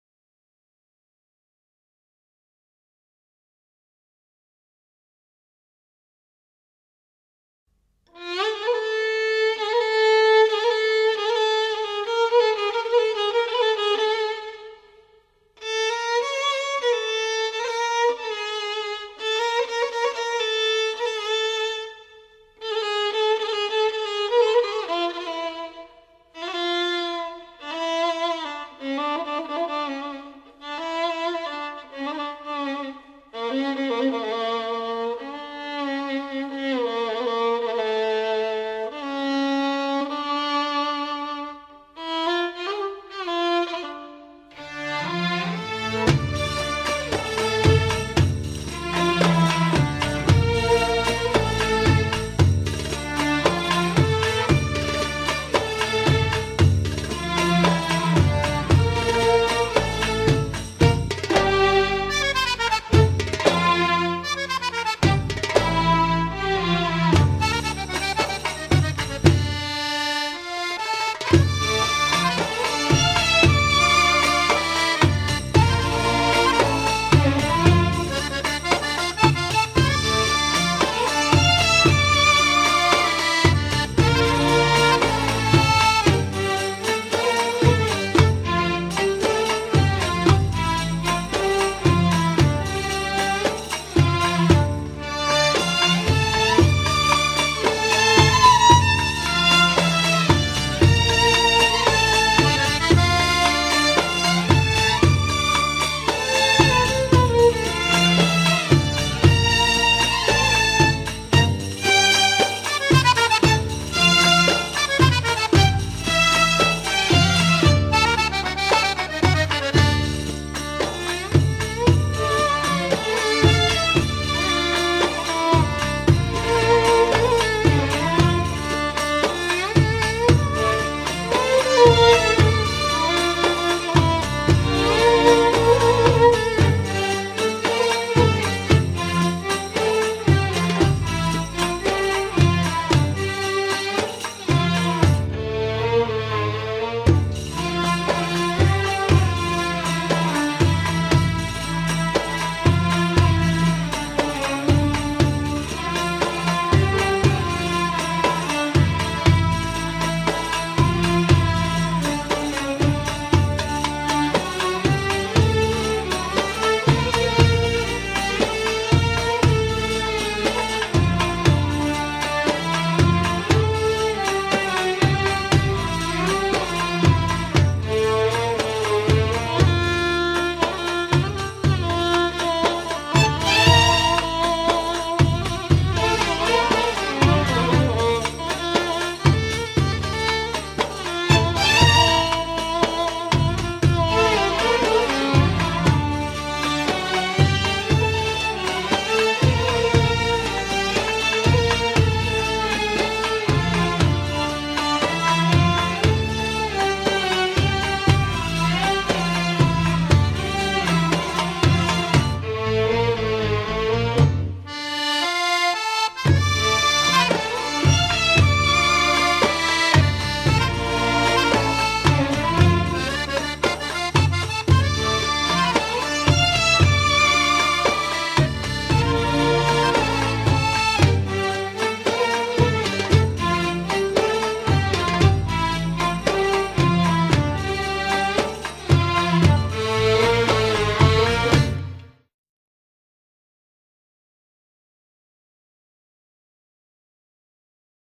Bolero Oriental
Trabajo realizado durante los últimos meses del 2020 en Curso Avanzado.